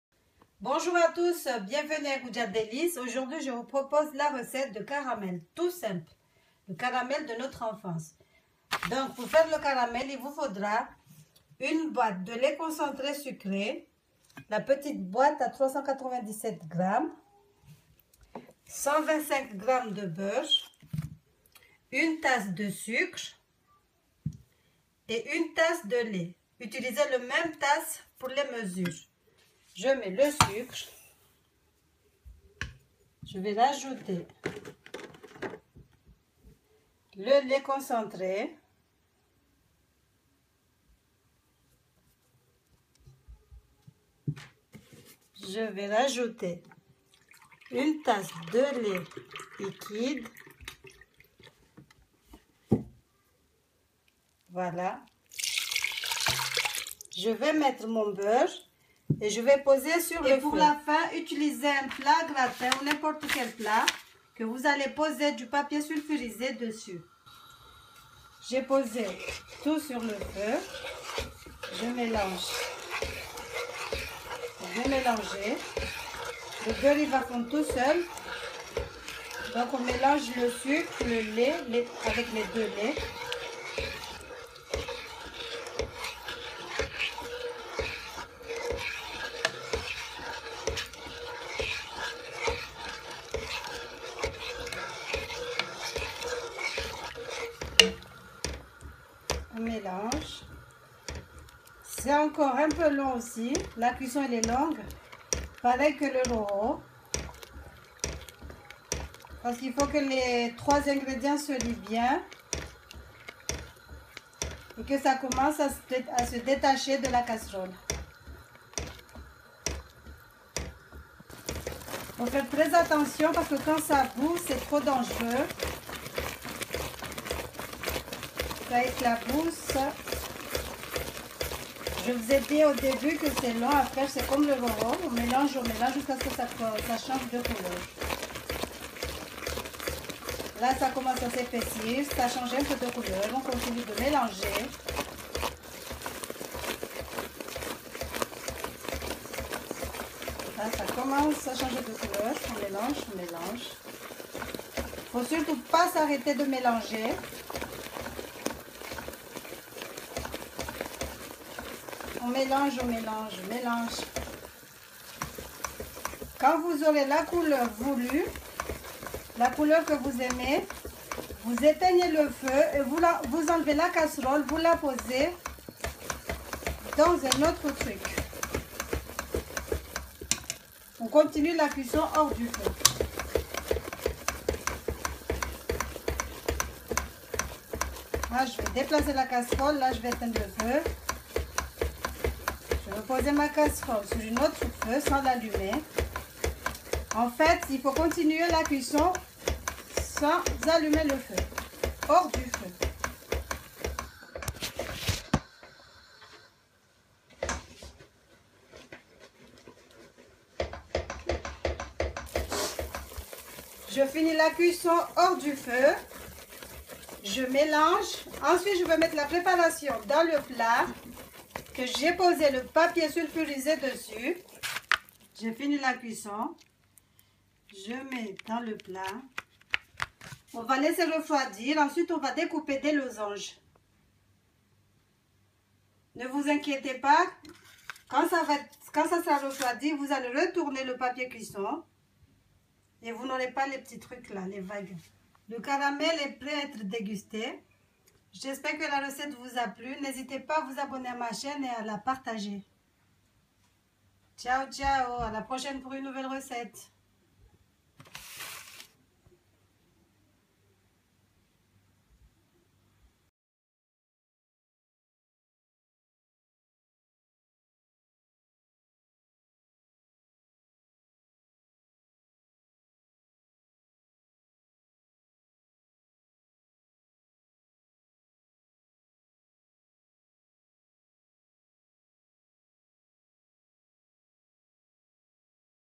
Caramel (mon enfance)
1 boite lait concentre sucre
1 tasse de sucre
1 tasse de lait demi ecreme
125g de beurre
Voud pouvez utiliser du beurre sale